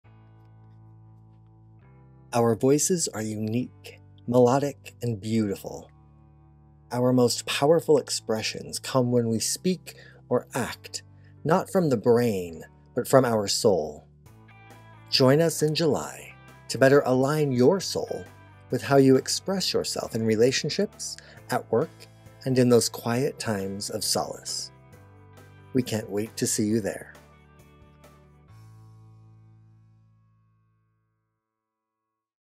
Your Results from Attending the July 2024 Retreat
Source-Centered Living

Our voices are unique, melodic, and beautiful. Our most powerful expressions come when we speak or act, not from the brain but from our soul. Join us in July to better align your soul with how you express yourself in relationships, work, and quiet times of solace. I can’t wait to see you there.